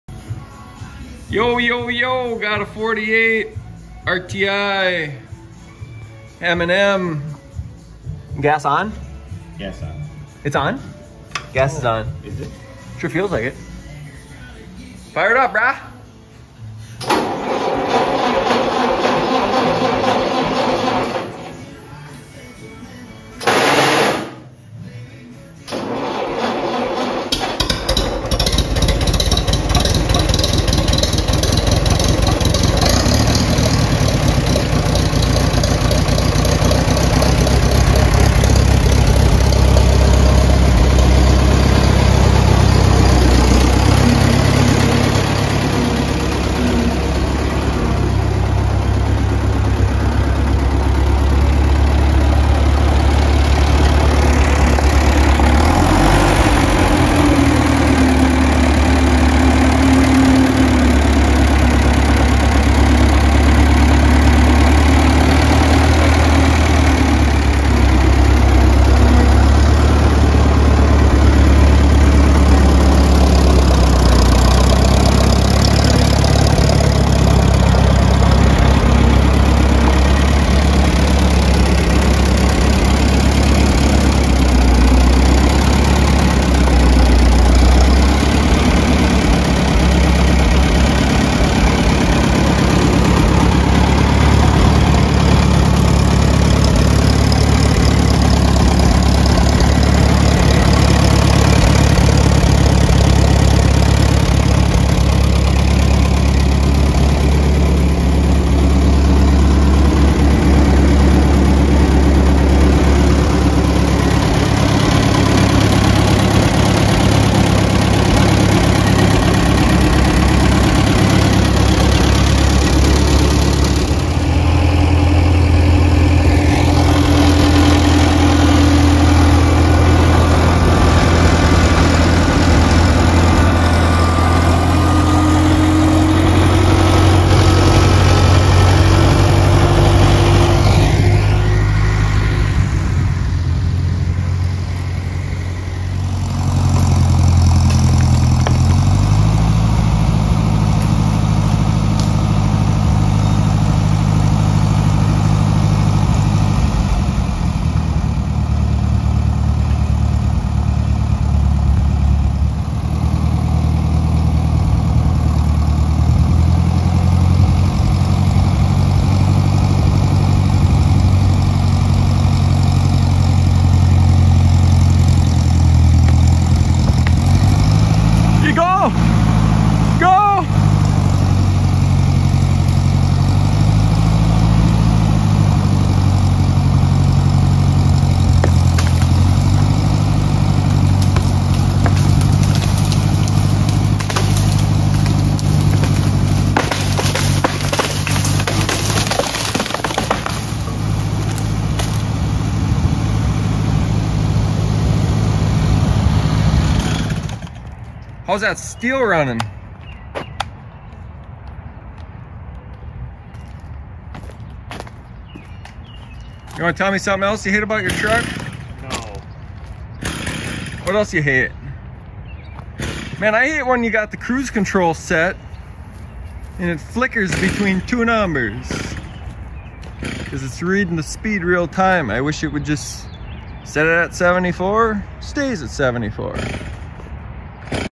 1948 Minneapolis Moline RTI Pulls Down Dead Tree
My buddy Tim has the tractor and it's nice to see him doing tractor things with it. Brap brap brap.

How to pull down a dead tree without a chainsaw. :)